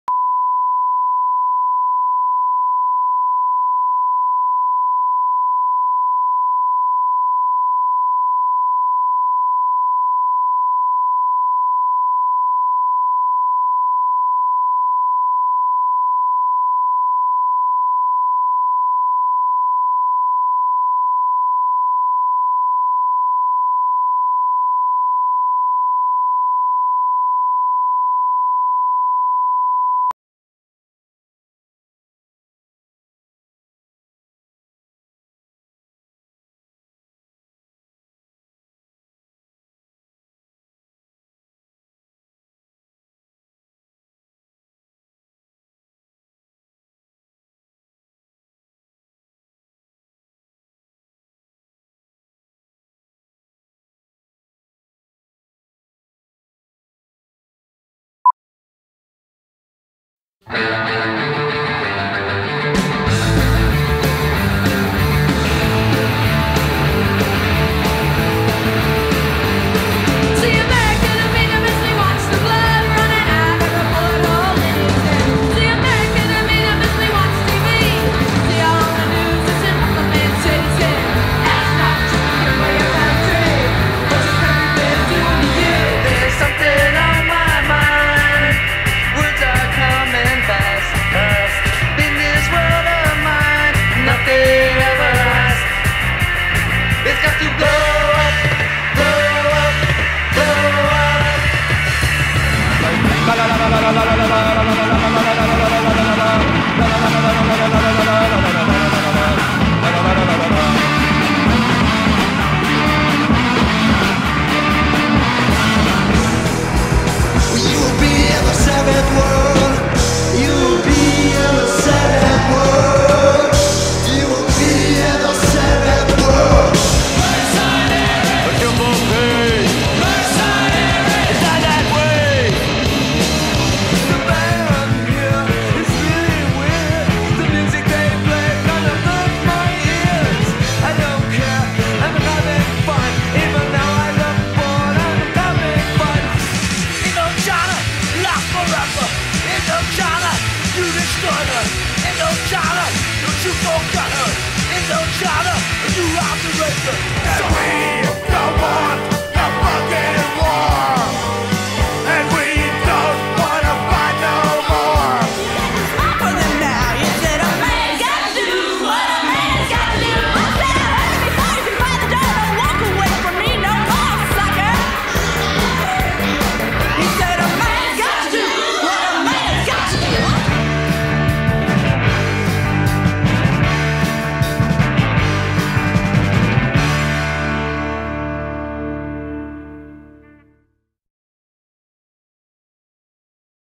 SF Punk trailer
Snapshots of pioneering Bay Area punk bands by J Neo Marvin, accompanied by their music. Edited for the BECA program at SF State, Fall 2013.